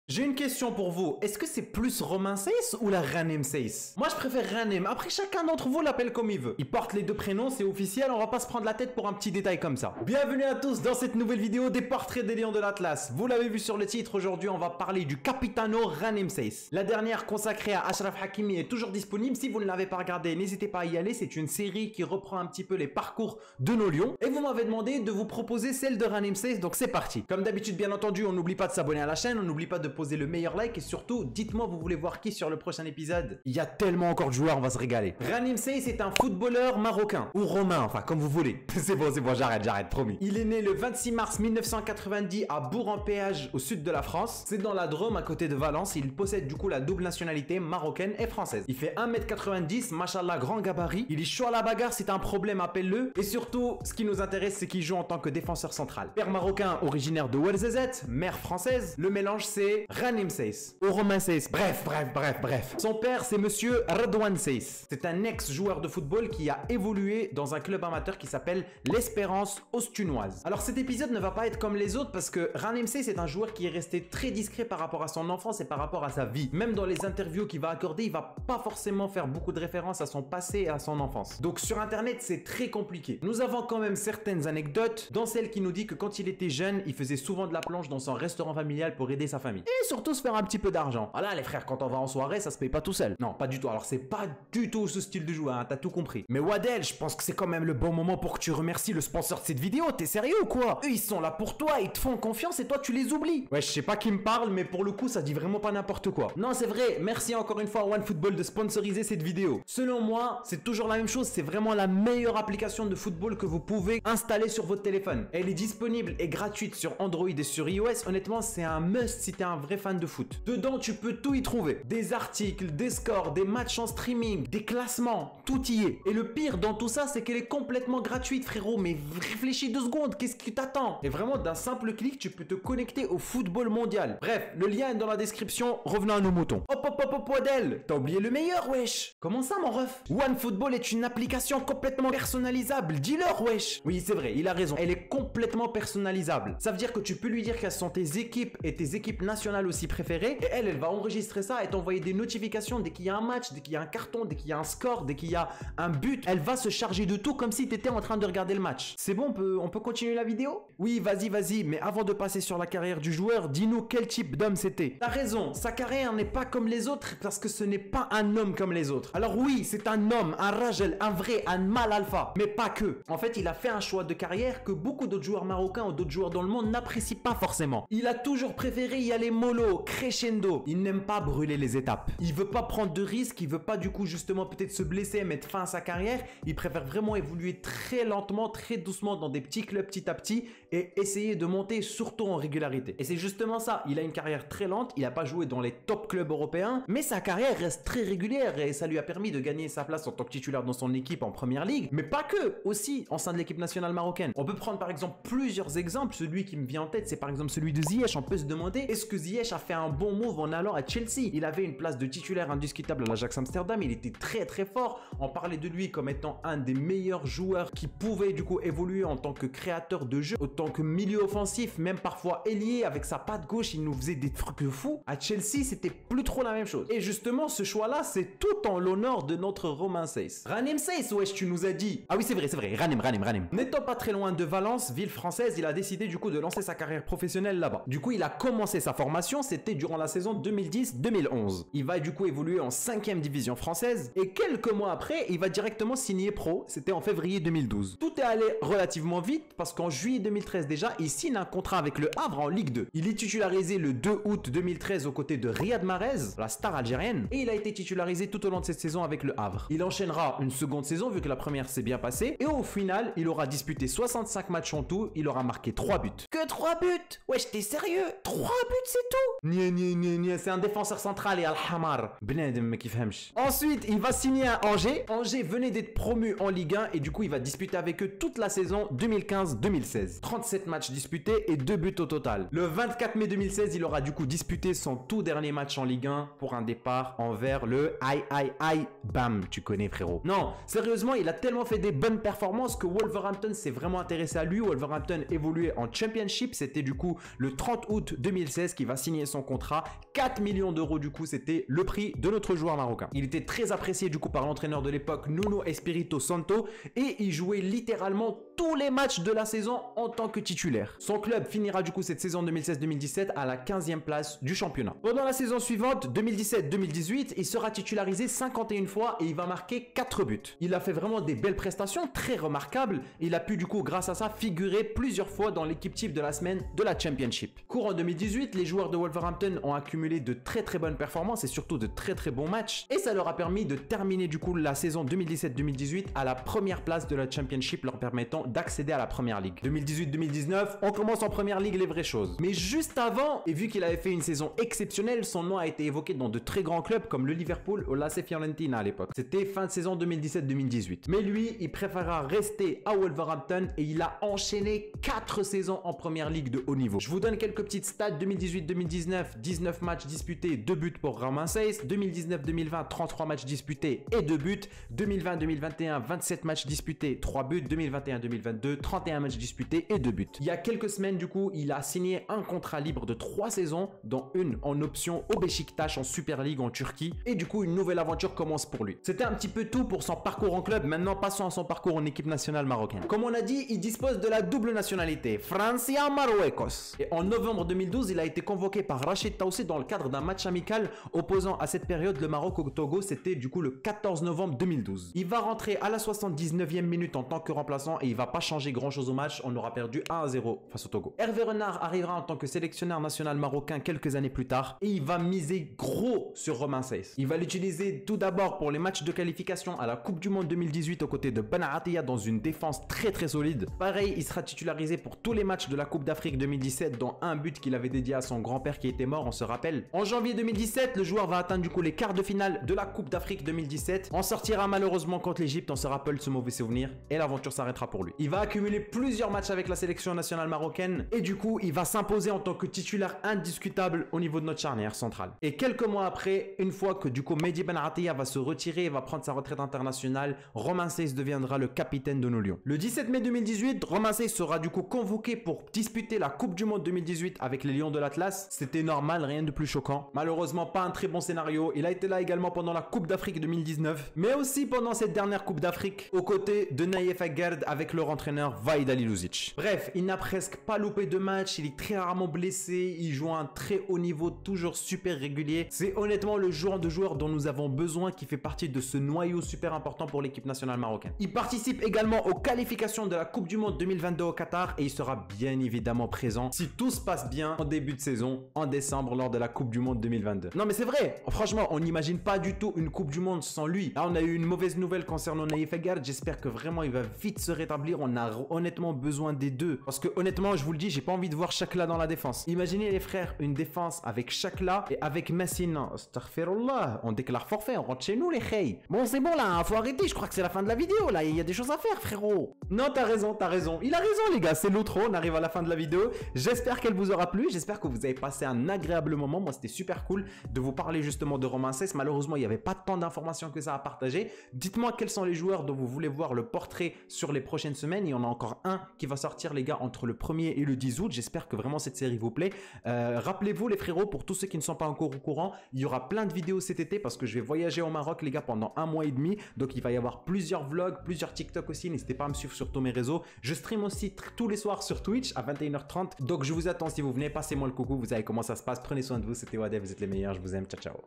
LE PORTRAIT DE ROMAIN SAISS (de Défenseur de Ligue 2 à Capitaine du Maroc)
LIKE SI TU VEUX PLUS DE VIDEOS SUR CETTE SERIE

Bonjour tout le monde, nouvelle vidéo aujourd'hui, on découvre le parcours de Romain Saiss, un capitaine pas comme les autres.
Je vous laisse découvrir ça en vidéo, je vous retrouve très bientôt pour une prochaine, prenez soin de vous, peace.